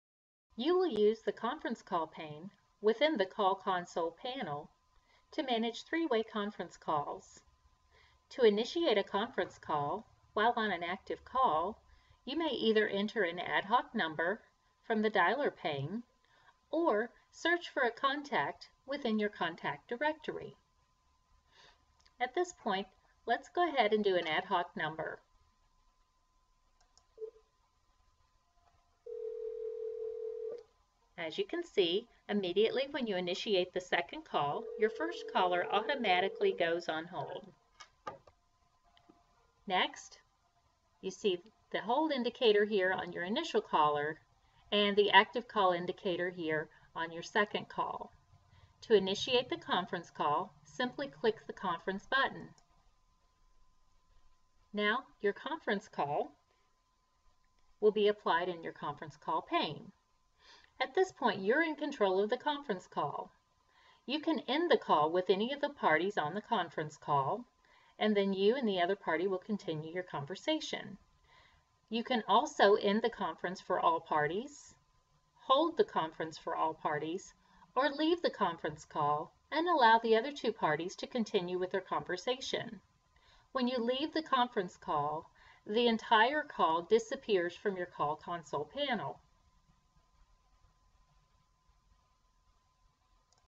Connected Office Voice Tutorial - Receptionist Conference Calls
This video contains details on how to initiate a conference call, place the conference on hold, and also how to drop a specific party from the conference call.